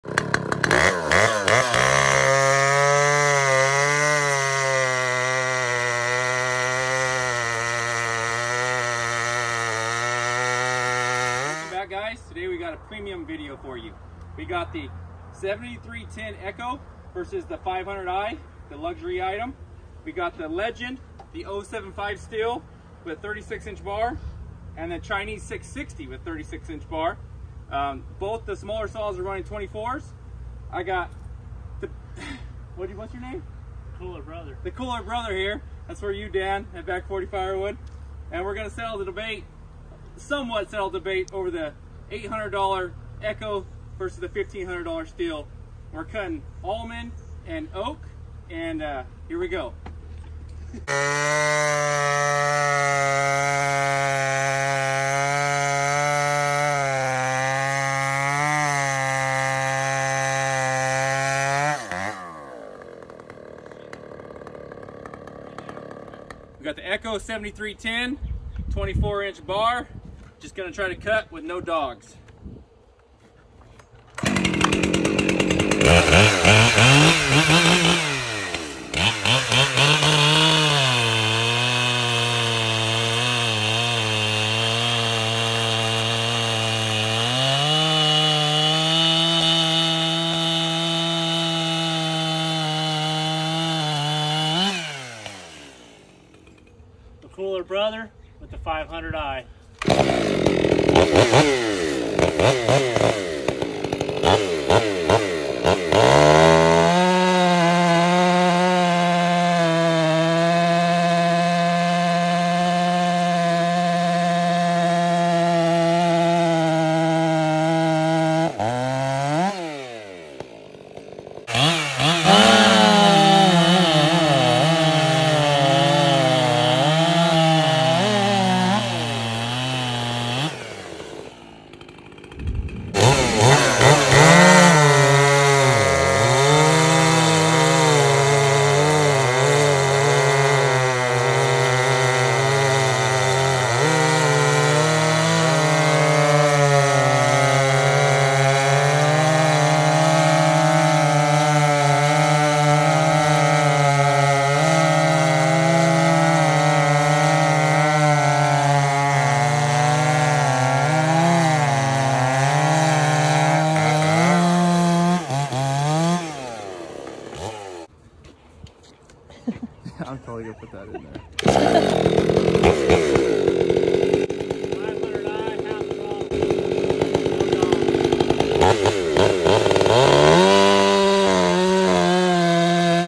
Stihl 500i vs Echo 7310 and the big 90cc+ class.
Having some fun comparing saws.  Do you need to break the bank for a saw!? Or are the value brands a contender……@Back40Firewood